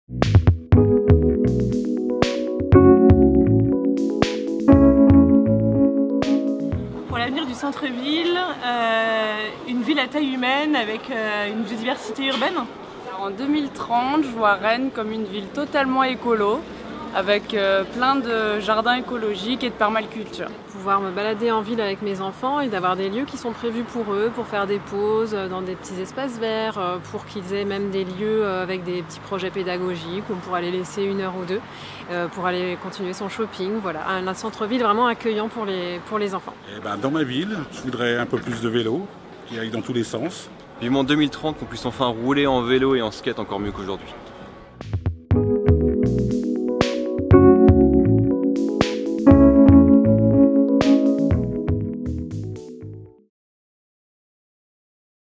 Rennes 2030: paroles d habitants sur le coeur de ville
Que souhaitent les habitants pour le coeur de Rennes, en 2030? Quels visages, quels services, quels déplacements... La concertation continue de septembre à décembre 2018.